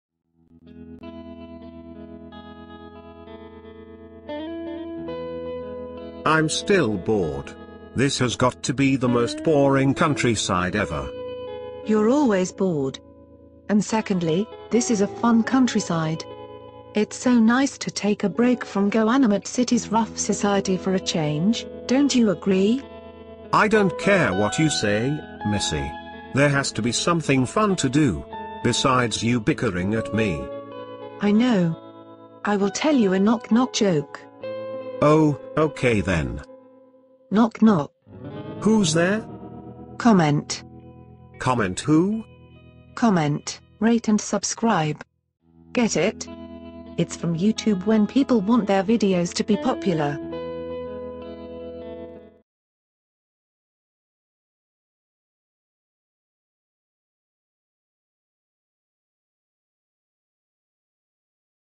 A knock knock joke at the GoAnimate countryside
The pumpkin guy thinks the GoAnimate countryside is the most boring countryside ever, but his friend decides to entertain him with her latest knock knock joke.